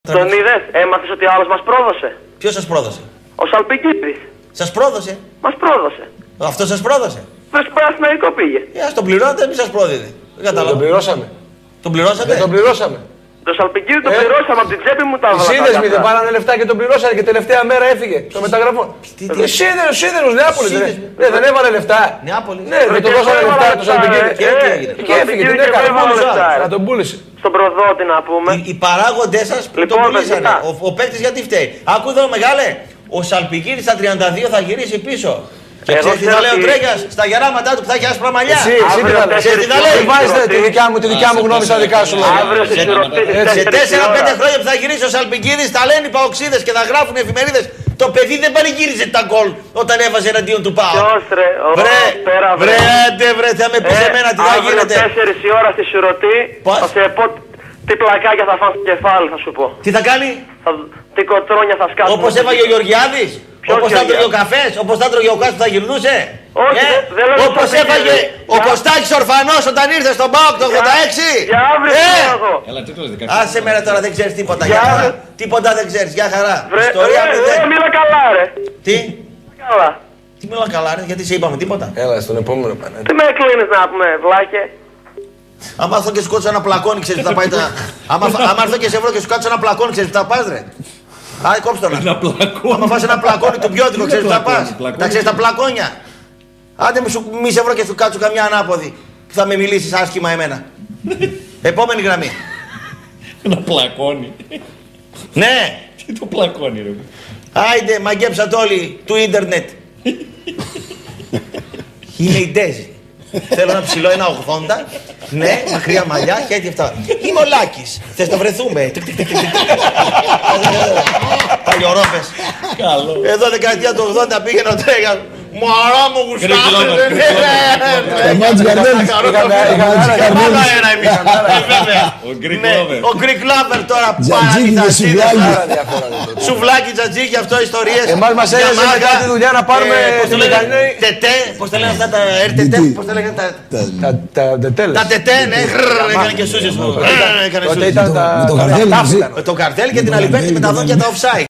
Ο ΠΡΟΔΟΤΗΣ ΣΑΛΠΙΓΓΙΔΗΣ,ΤΑ ΤΟΥΜΠΙΩΤΙΚΑ ΠΛΑΚΩΝΙΑ ΚΑΙ Ο ΜΟΝΟΛΟΓΟΣ ΚΑΤΑ ΤΗΣ ΤΕΧΝΟΛΟΓΙΑΣ
ΕΙΜΑΙ Η ΝΤΑΙΖΗ ΘΕΛΩ ΕΝΑΝ ΨΗΛΟ 1,80 ΜΑΚΡΥΑ ΜΑΛΛΙΑ ΧΑΙΤΗ ΚΑΙ ΑΥΤΑ
ΕΙΜΑΙ Ο ΛΑΚΗΣ, ΘΕΣ ΝΑ ΒΡΕΘΟΥΜΕ? ΤΙΚΤΙΚΤΙΚΤΙΚΤΙΚ